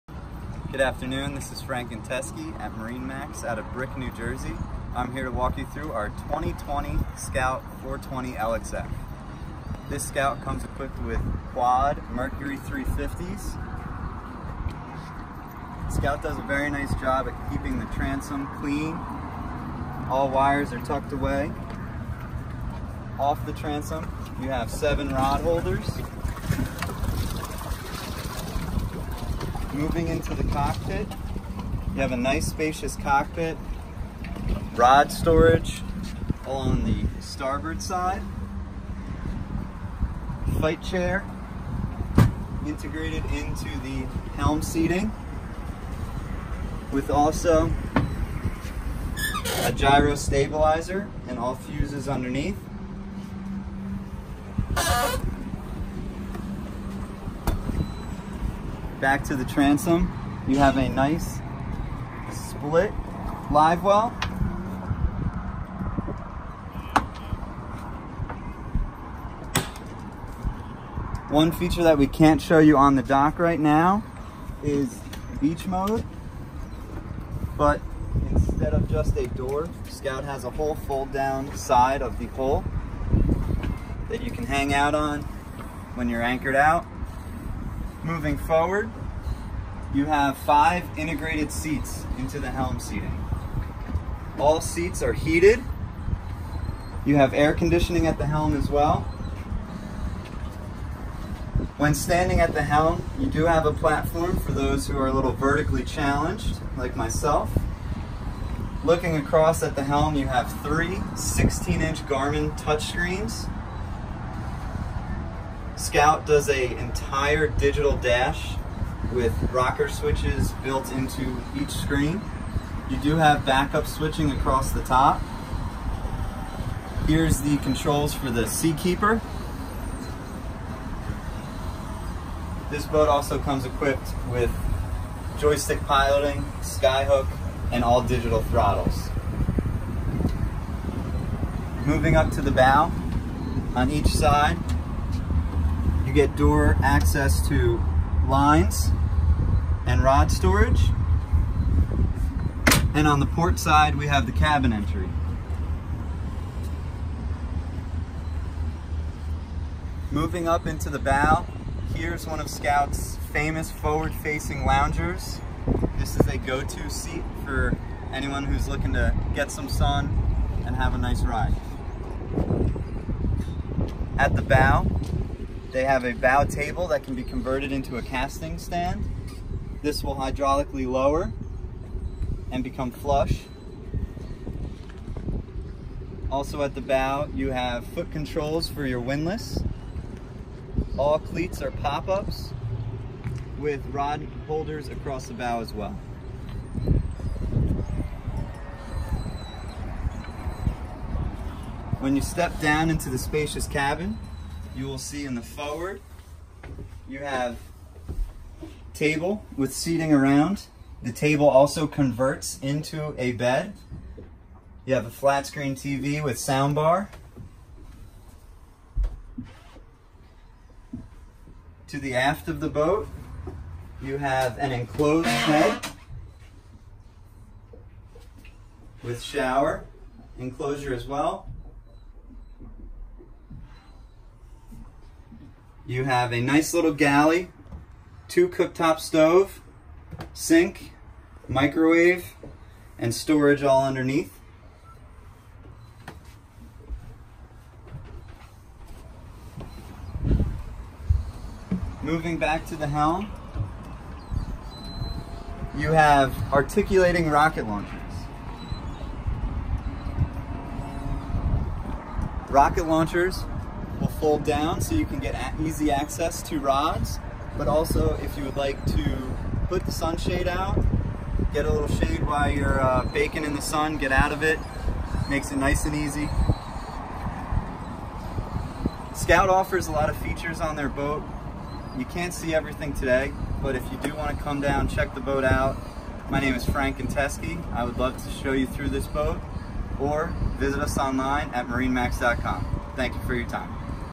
2020 Scout 420LXF For Sale at MarineMax Brick
The 420 LXF is a landmark model for the marine industry. No stone was left unturned in the design and development of this beauty. It’s Epoxy-Infused hull provides improved structural stability and reduces the overall weight of the boat, combining for unsurpassed performance. Unique features never before seen on a model like this…on second thought…you’ve never seen a model like this before, period.